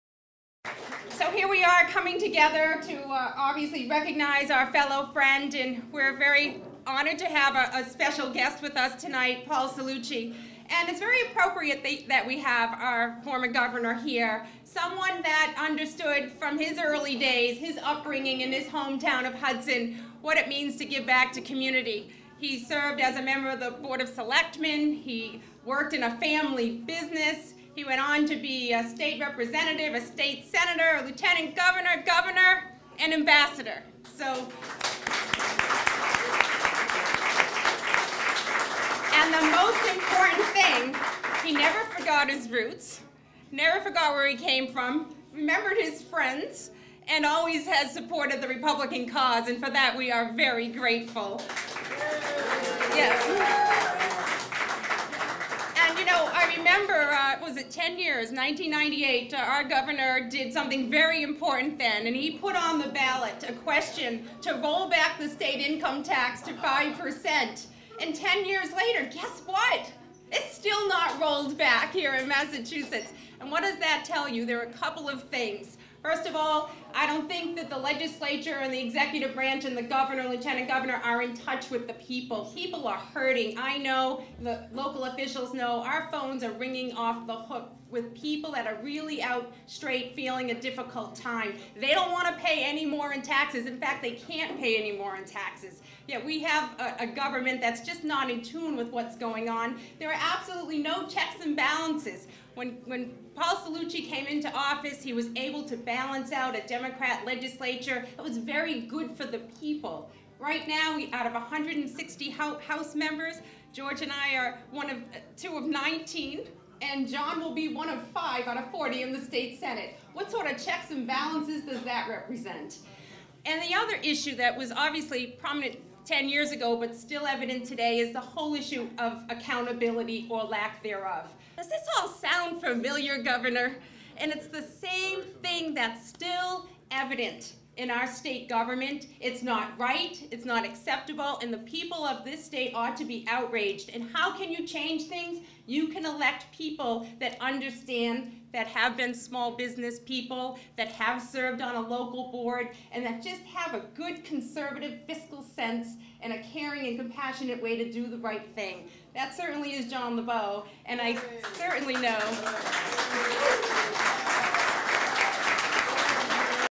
Karyn Polito Remarks at October Lebeaux Rally
Kayrn Polito introduces former Massachusetts Governor Paul Cellucci